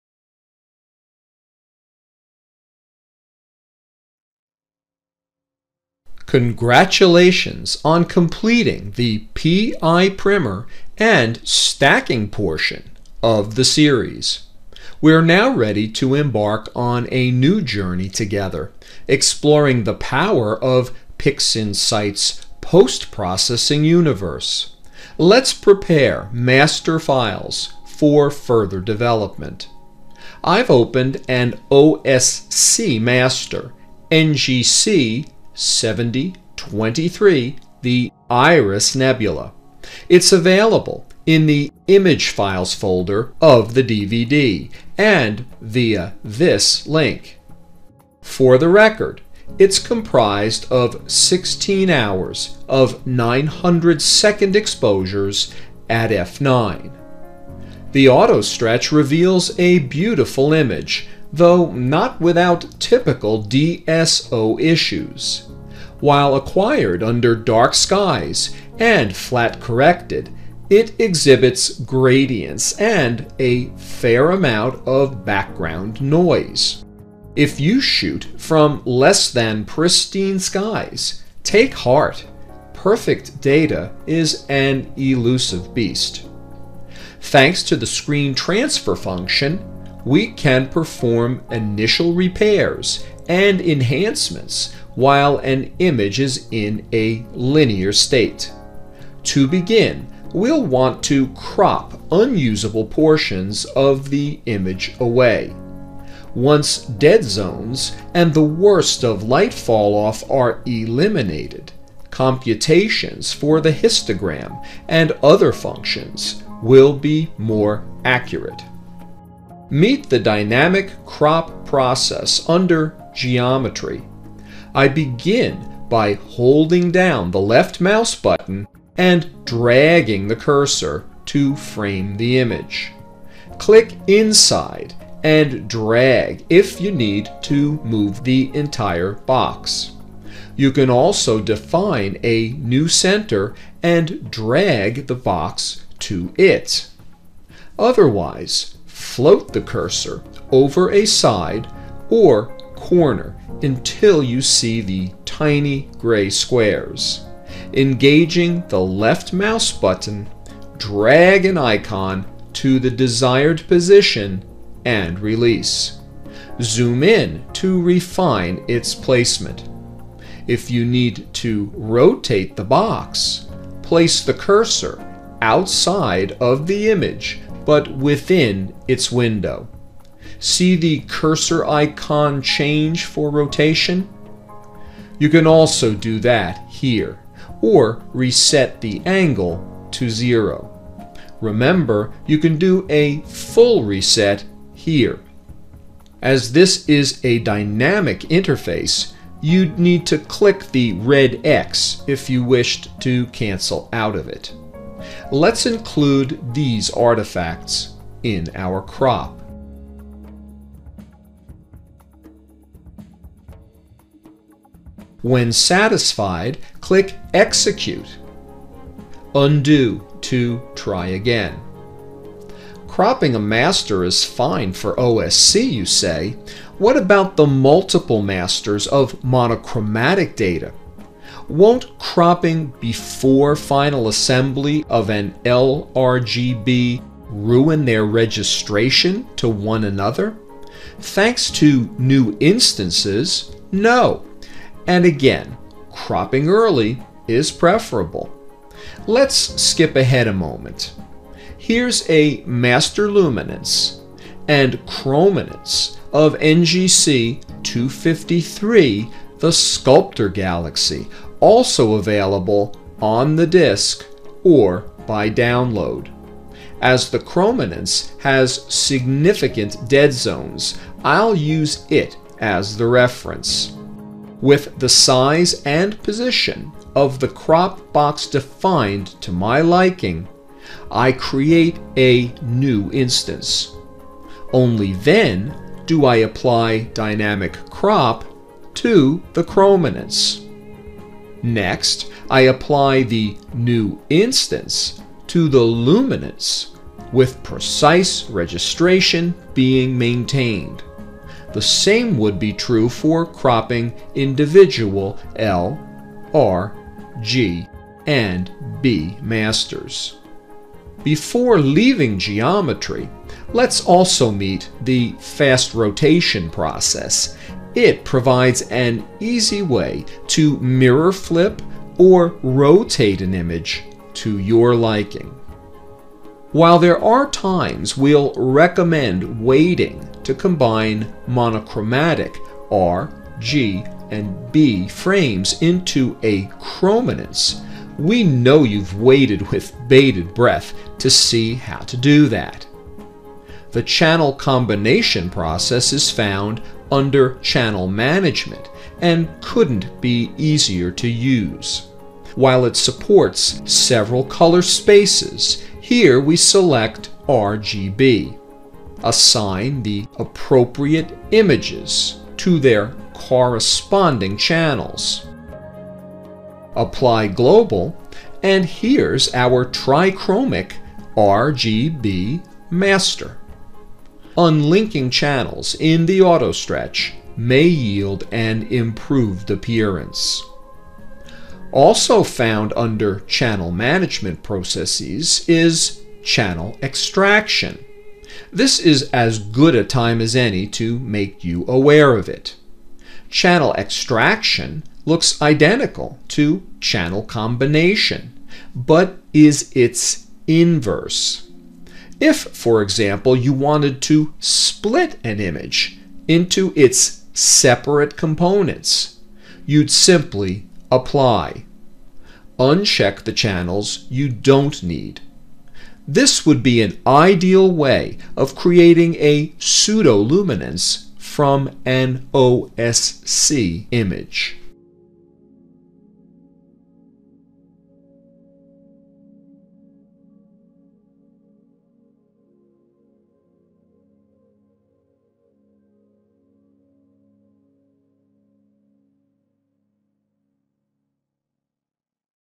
PixInsight Part-1 PI-14 Linear-1
Linear-1 Postprocessing in the linear state. The DynamicCrop, FastRotation, ChannelCombination, and ChannelExtraction processes are introduced